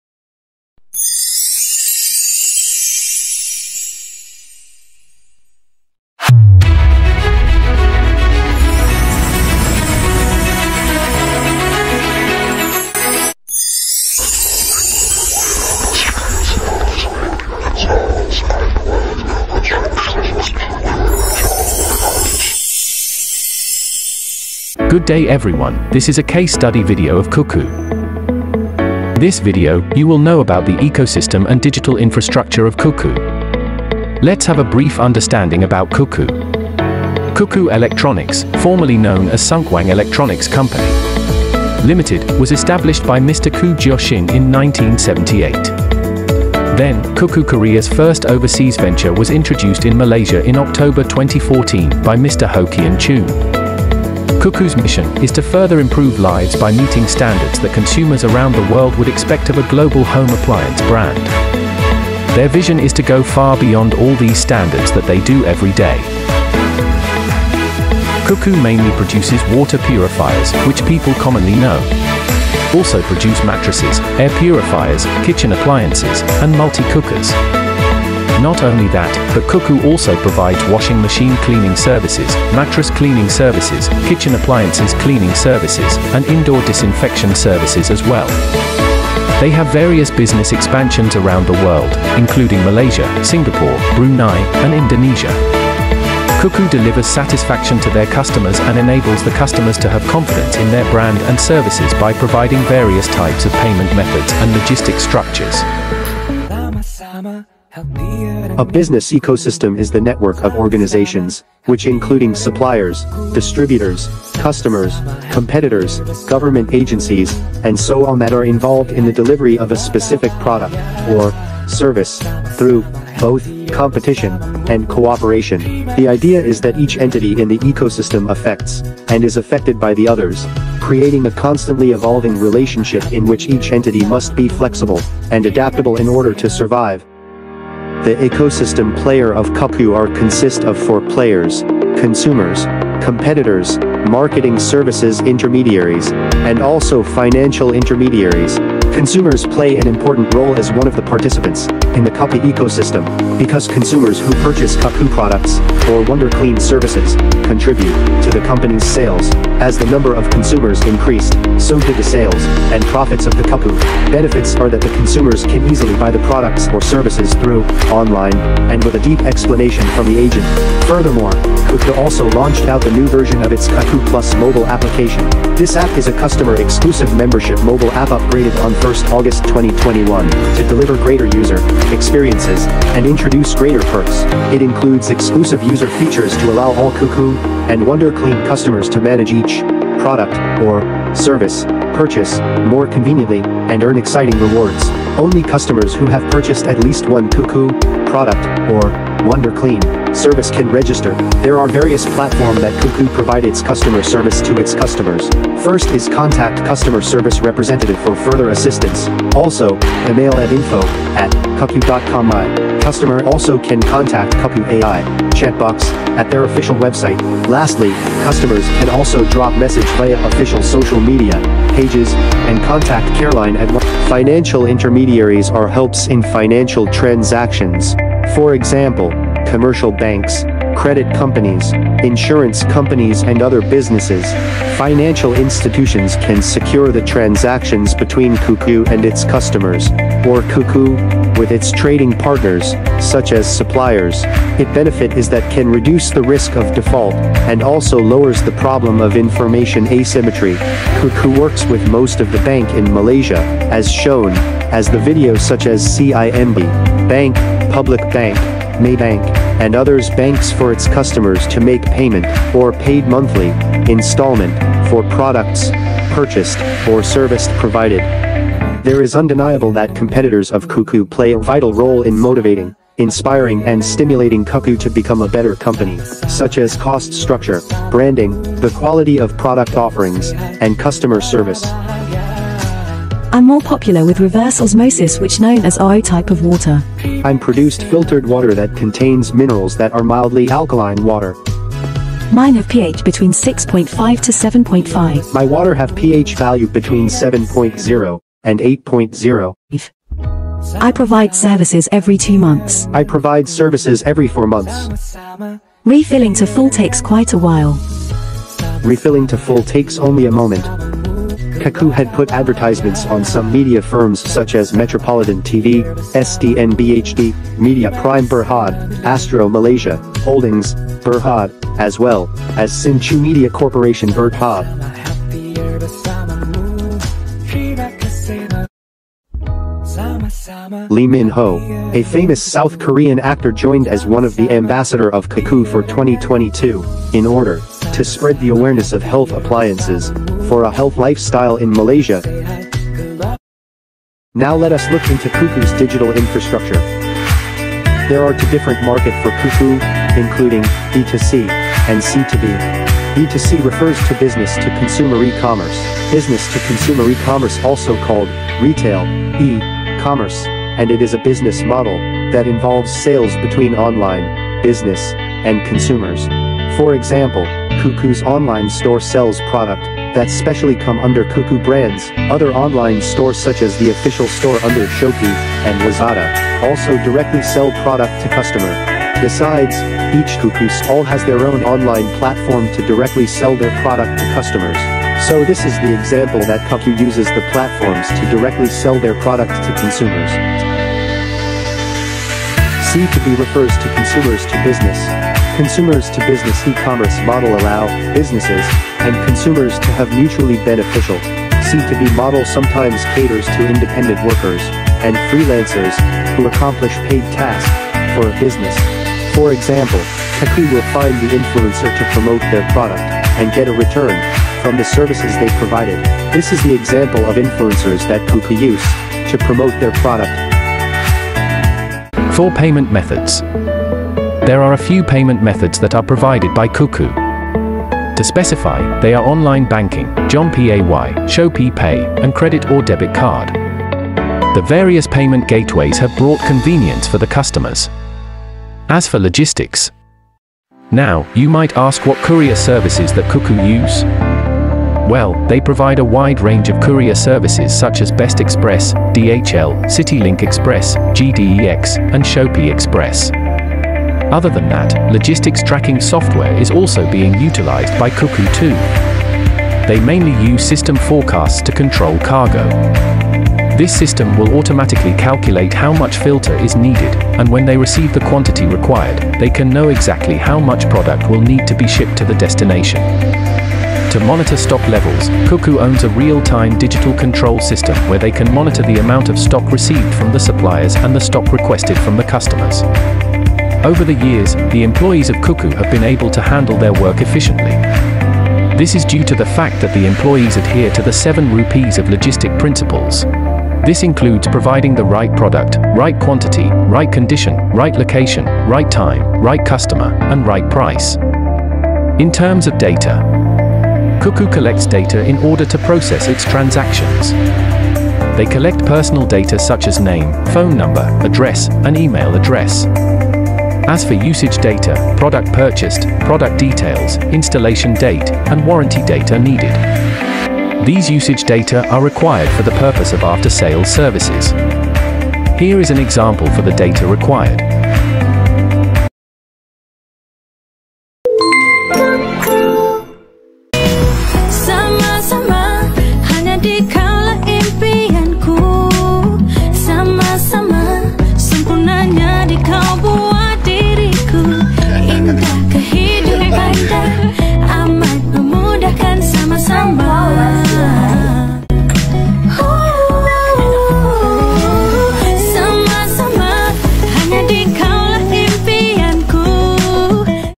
Introduction to the Digital Economy - Case study of Cuckoo｜UBAI3113 Applied E-Commerce
Hello everyone, we are students of UBAI3113 Applied E-Commerce from University Tunku Abdul Rahman (UTAR). This is a case study on the introduction to the Digital Economy: Inclusiveness. For our group assessment, we have selected to conduct and in-depth case study on Cuckoo.

Video produced by: P8G2
Chow Sue Nee
Lee Xue Ling
Nie Naik Jia Yi
Toh Khang Yun
Yip Chiaw Ling

Lastly, we truly appreciate for the time and effort that was contributed by our interviewee (Mr.Mo, Madam Lee and Ms. Liew).

Most importantly, we like to express a special thanks to our course lecturer and tutor, Dr Mohd Nizam bin A. Badaruddin for guiding us with great patience upon completing this case study.

Hope you guys enjoy the video and have a nice day! Please leave us with a thumbs up and a positive comment.